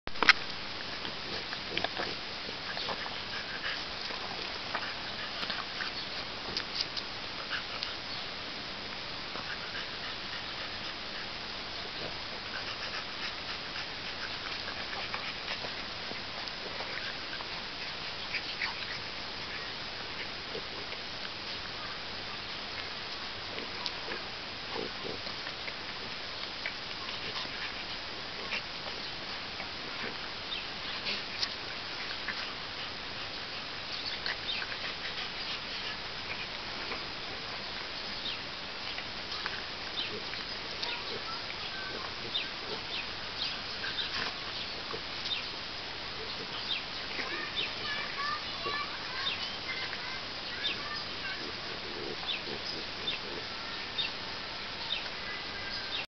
Happy french bulldogs
9 weeks old French bulldog puppies play with their friend